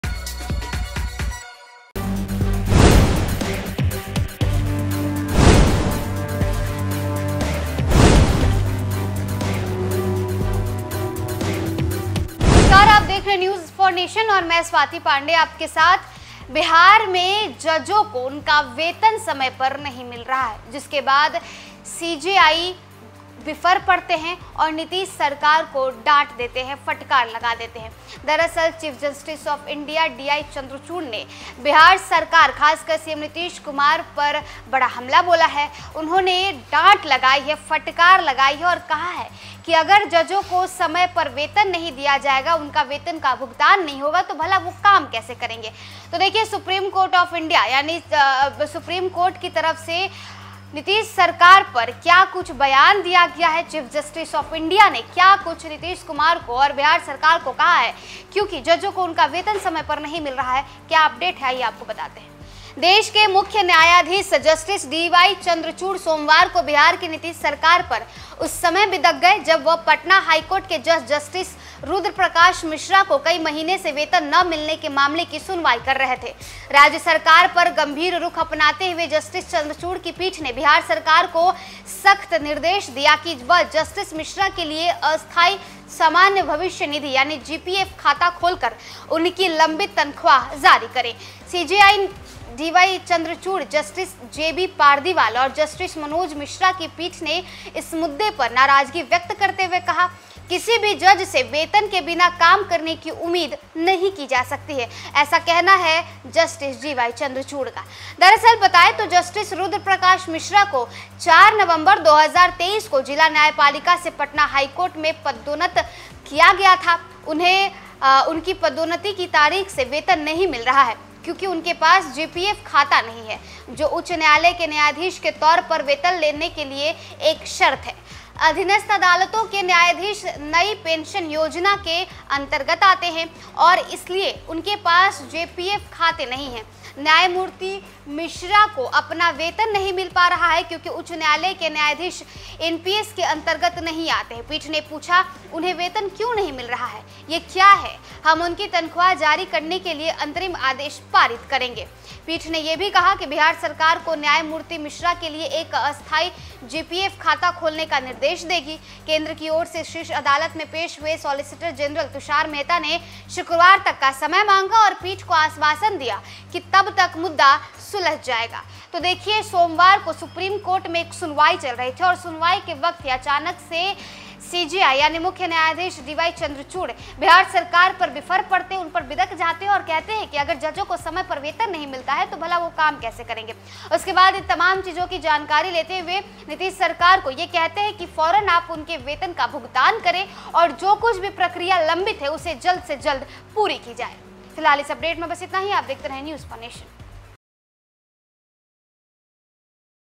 Nitish Kumar पर भयानक भड़के CJI Chandrachud, Supreme Court में ही सुना दिया
Nitish Kumar  पर भयानक भड़के CJI Chandrachud, Supreme Court में  ही सुना दिया | Bihar News | News4Nation